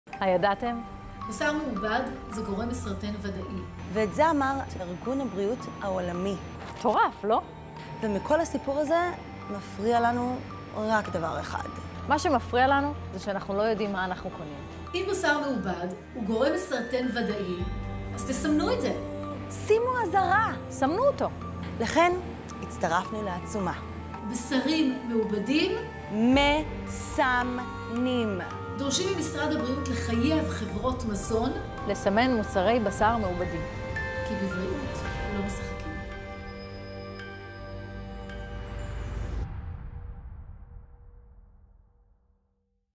מאבק האמהות לסימון מוצרי בשר מעובדים!
אם בשר הוא גורם מסרטן וודאי - סמנו אותו!
מיכל צפיר, עדן הראל וליהי גרינר יוצאות בדרישה אמיצה וחשובה לסמן מוצרי בשר מעובדים, עבורן ועבור מי שיקר להן מכל - הילדים שלהן.
זהו מאבק של כולנו!